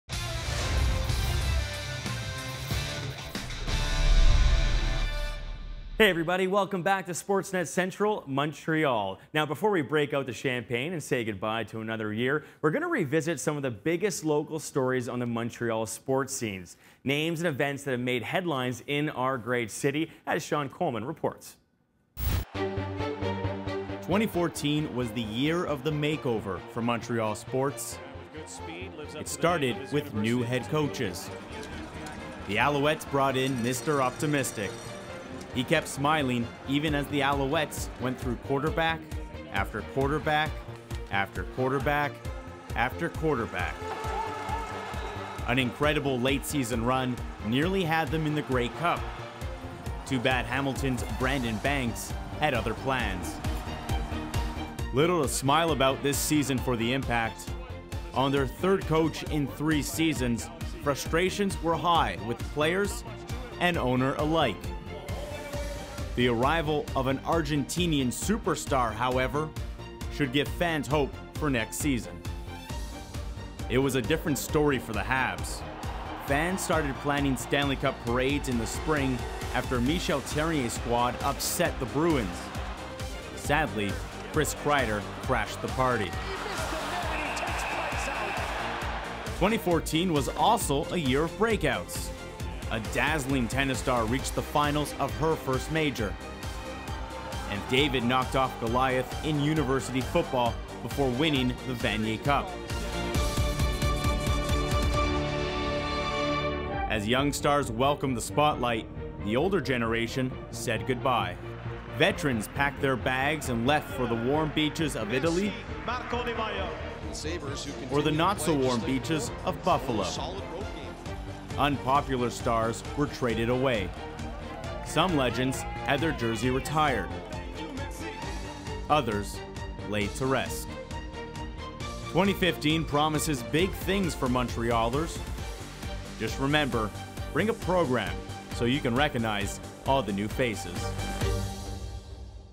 2014 in Montreal Sports
It was a year of the highest highs for athletes like Eugenie Bouchard and lowest lows for teams like the Impact. We look back at the year that was in Montreal sports.